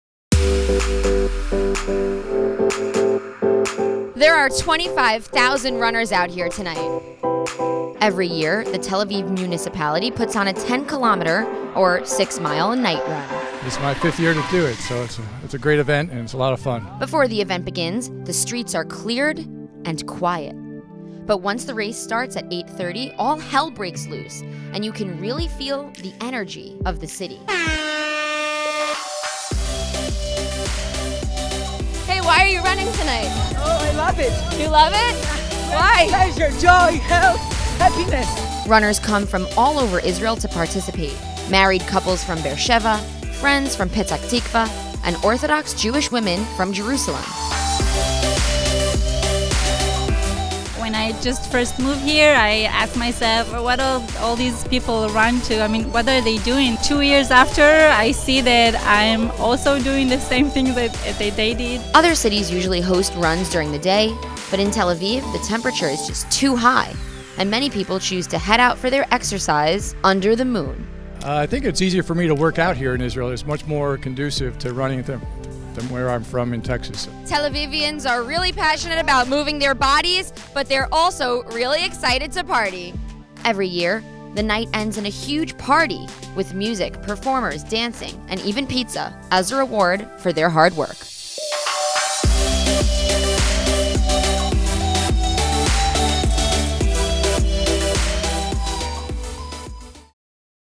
Tel Aviv Night Run returns with 25,000 participants
Over 25,000 runners participated in the annual Tel Aviv Night Run. ILTV's Shanna Jacqueline Fuld went down to the streets to check it out.